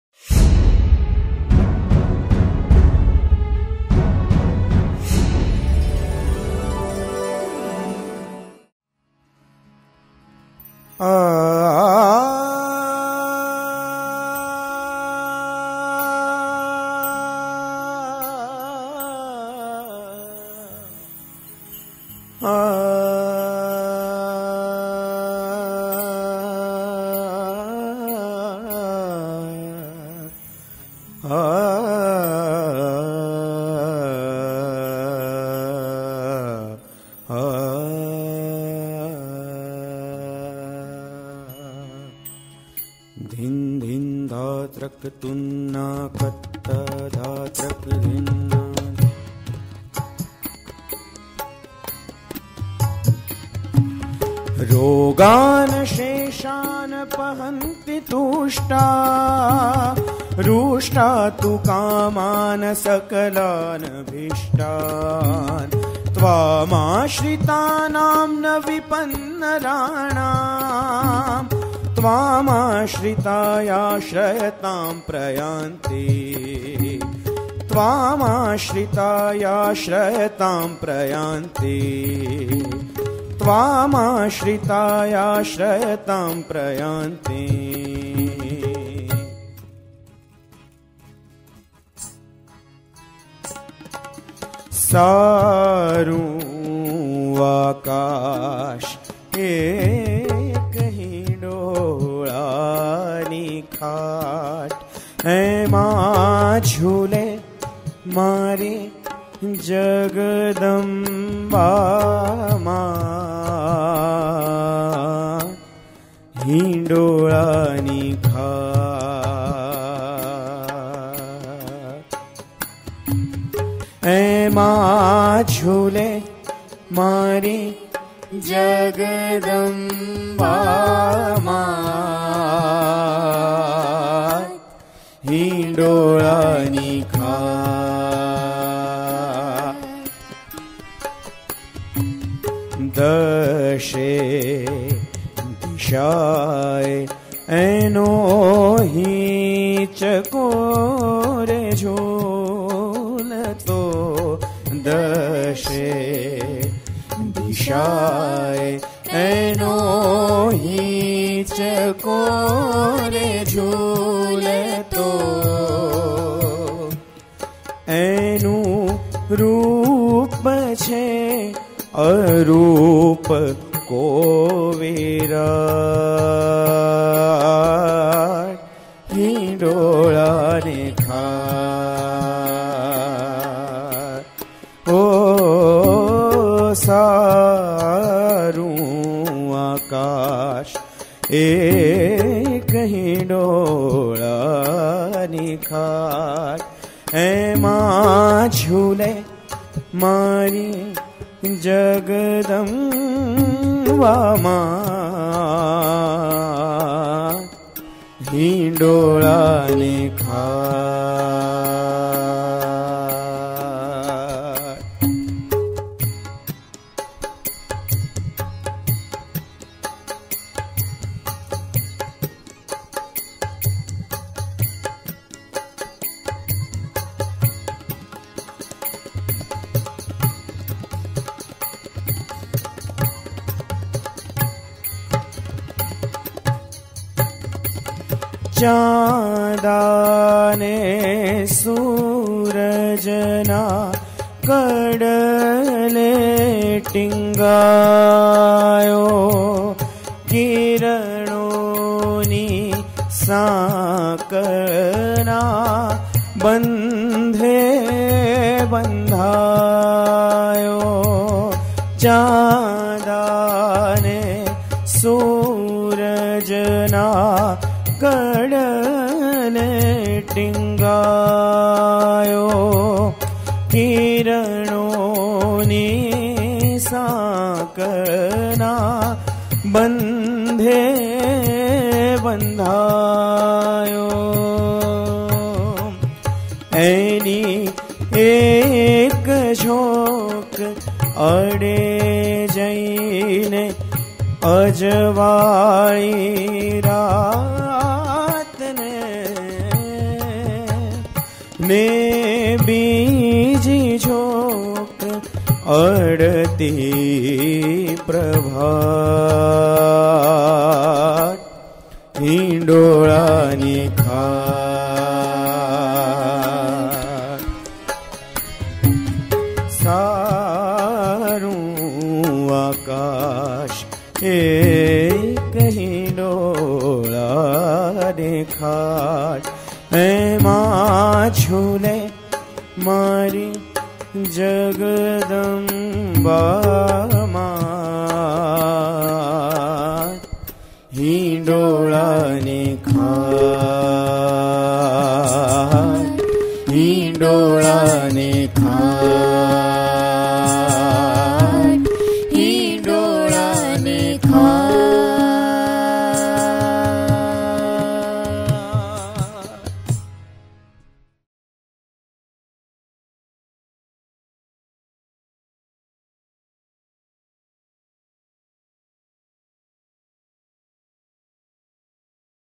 Jalso | Praher & Sampada Vora | Stuti | Live Jamming (Solo Track)
જગદંબા માં નો સુંદર ગીત સાંભળો
Watch the YouTube Music Video of Praher Vora and Sampada Vora, one of the finest singing pairs in the Gujarati Music Industry in Jalso Live Jamming. You can listen to their Live Jamming Session with best audio quality on Jalso application.